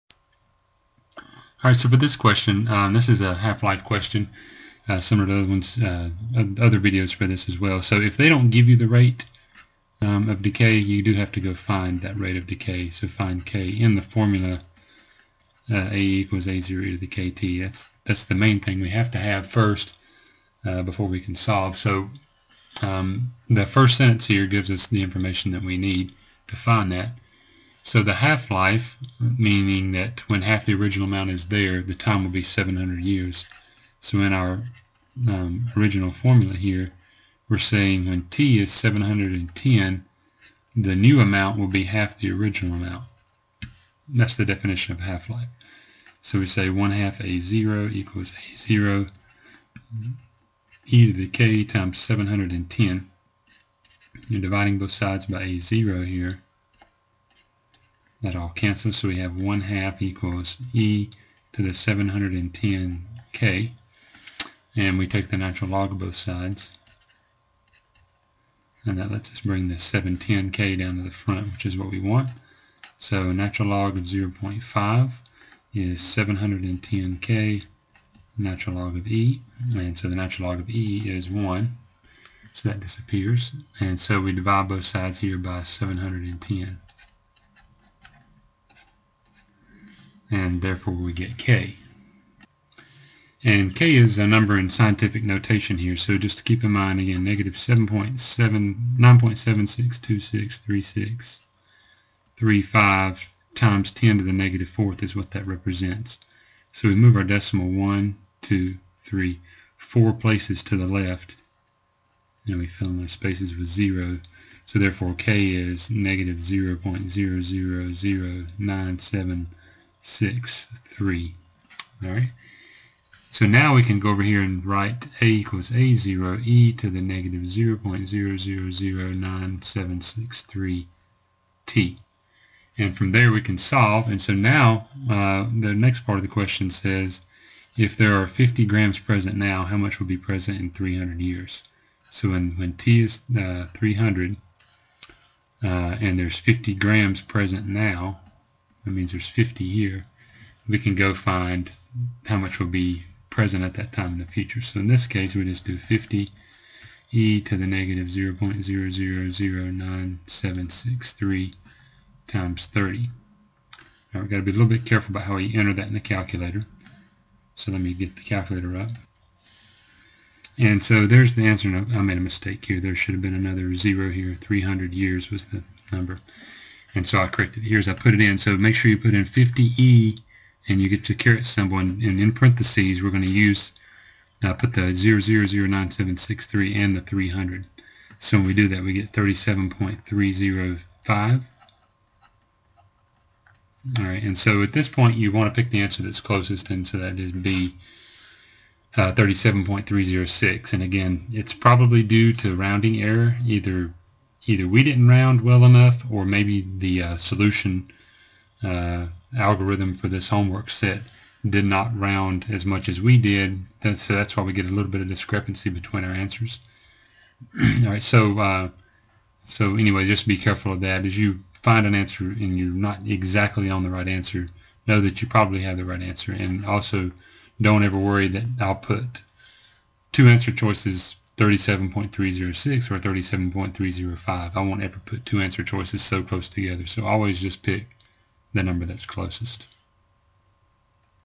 Exponential Growth/Decay - half-life of silicon-32
This video shows another half-life word problem, but the latter part of the solution is a bit simpler than the carbon-14 example.

TestGen, Blitzer, College Algebra